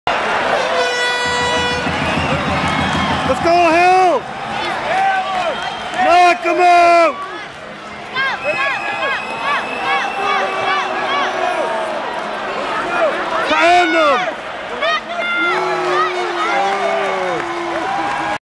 Jordon Hill Vs McKelvie Game 3 St. John's Ice Caps Vs Albany
Kael Mouillierat scored the only goal of the game and Michael Hutchinson stopped 24 shots to earn the shutout as the St. John's IceCaps blanked the Albany Devils, 1-0, in Game 3 of their Eastern Conference quarterfinal series on Wednesday night at Mile One Centre.